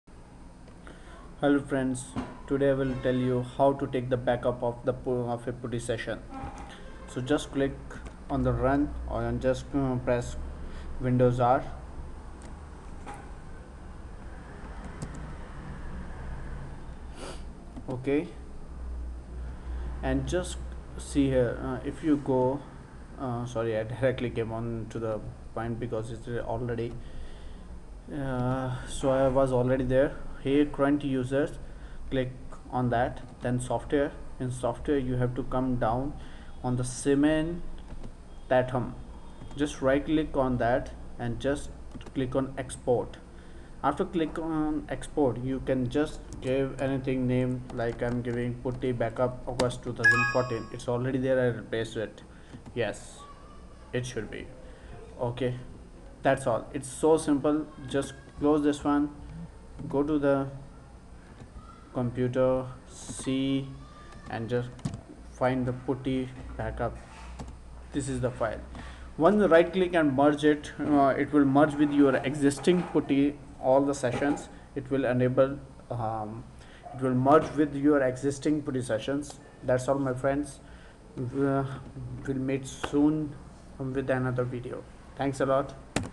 Putty backup
How to take backup of putty sessions